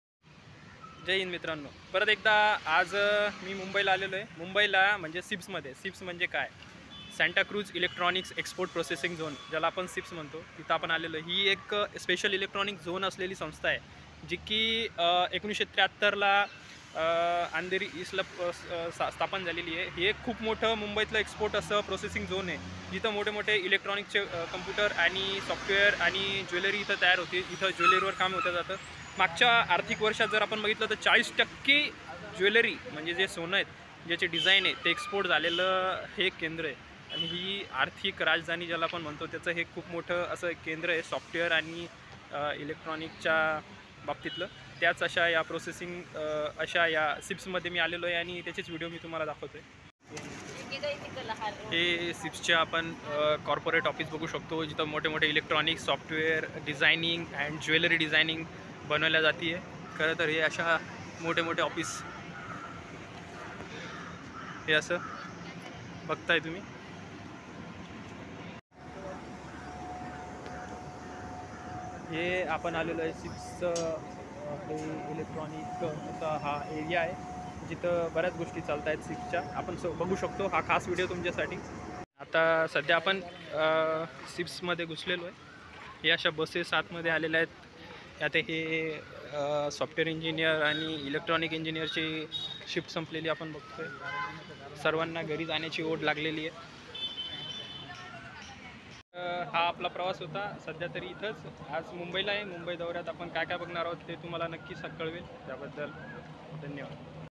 SEEPZ | SPECIAL ECONOMIC ZONE
Santacruz Electronics Export Processing Zone (SEEPZ) is a Special Economic Zone in Mumbai, India. Situated in the Andheri East area, it is subjected to liberal economic laws as compared to the rest of India to promote rapid economic growth using tax and business incentives and attract foreign investment and technology.